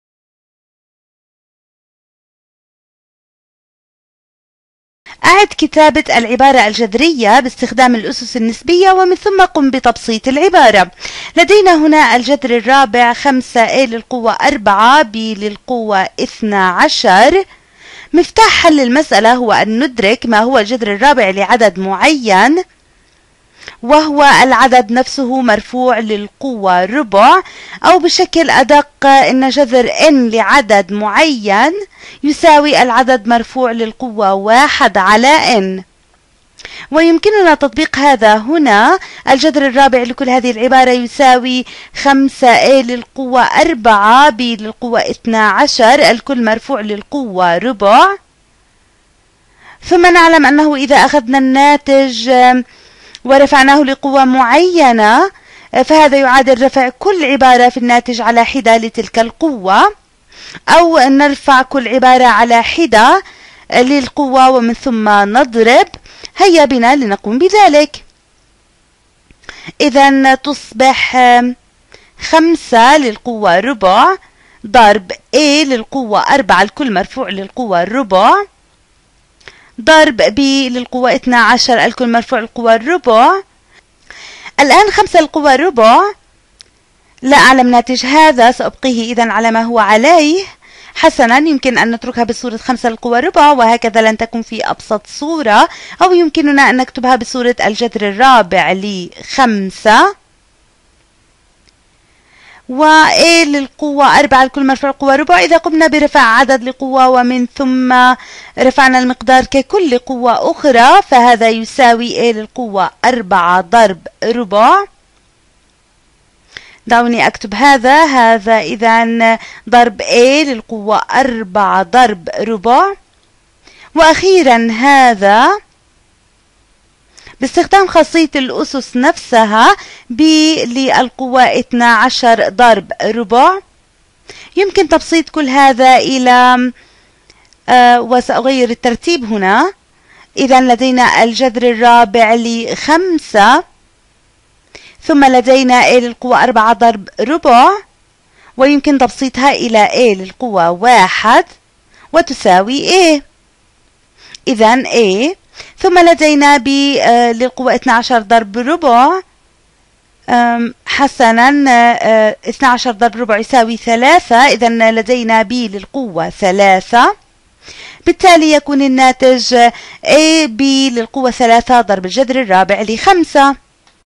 الجذر المكافئ للأسس النسبية 2
Radical Equivalent to Rational Exponents 2
يتناول هذا الفيديو مثالاً يوضح حل لعبارة جذرية من خلال القوة المكافئة للجذر
شكر خاص لمؤسسة شركاء في التنمية المستدامة -- فلسطين